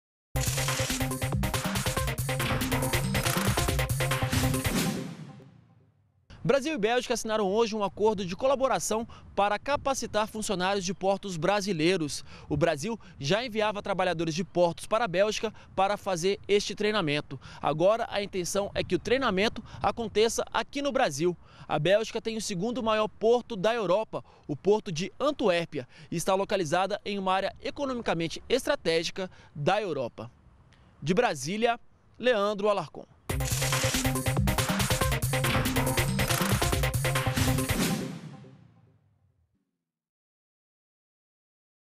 Brasil e Bélgica assinam acordo para capacitação de funcionários de portos brasileiros
ÚLTIMAS - 11.09.13: Brasil e Bélgica assinaram nesta quarta-feira (11) um acordo de colaboração para capacitação profissional de funcionários de portos brasileiros. O Brasil já enviava trabalhadores para a Bélgica para fazer esse treinamento. Agora, a intenção é fazer com que esse treinamento aconteça no país. A Bélgica possui o segundo maior porto da Europa, o de Antuérpia, que está localizado em uma área economicamente estratégica do continente.